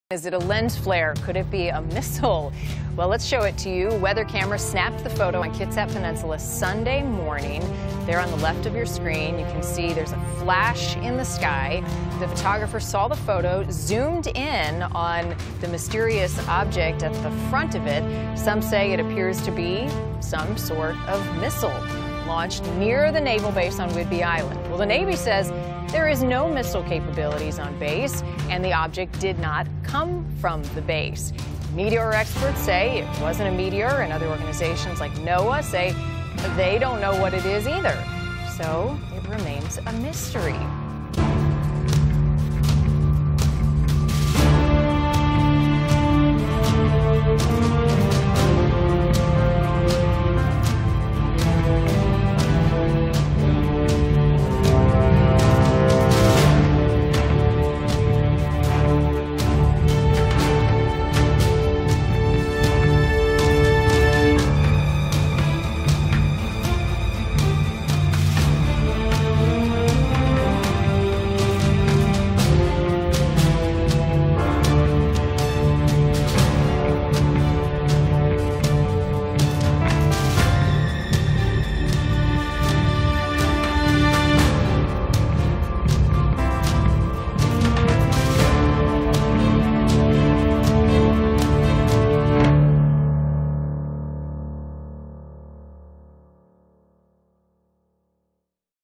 Mystery OBJECT Or SECRET Missile LAUNCH?
Greg Johnson of Skunk Bay Weather, a local weather website that runs camera enabled weather stations on the northern Kitsap Peninsula; Kitsap County, Washington, recorded a mysterious object early Sunday morning that has social media buzzing.  One of Johnson’s weather stations has a camera monitoring the Puget Sound at Whidbey Island from Skunk Bay, and at 3:56 a.m. Sunday by a high-resolution, 20-second exposure camera, snapped what looks like the impossible — a missile blasting off from what seems to be the Naval Air Station Whidbey Island.